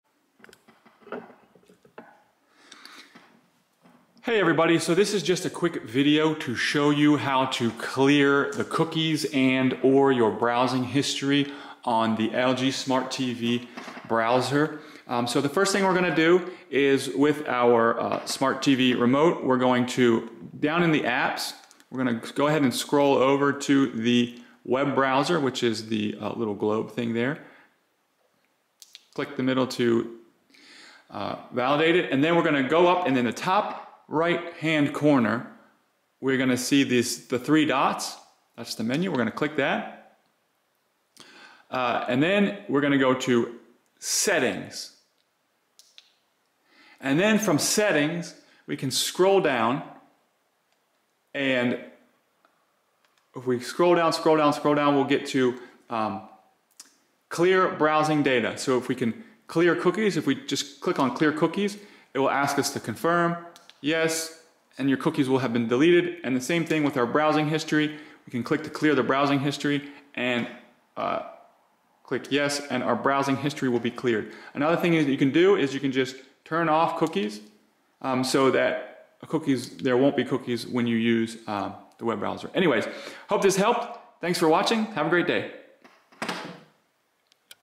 Delete Cookies and History on LG TV
How to delete or clear browsing history, cookies and cache on your LG Smart TV web browser.